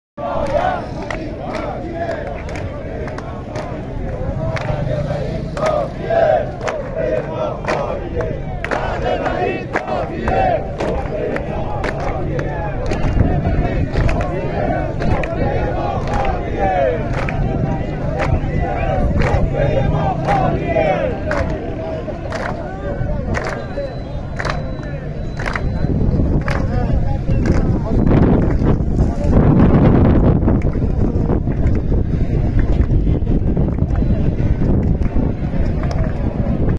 اراک - راهپیمایی اعتراضی بازنشستگان
بازنشستگان اراک هم ضمن برپایی تجمع اعتراضی با حمل سفره خالی شعار دادند:
 وعده وعید کافیه  سفره ما خالیه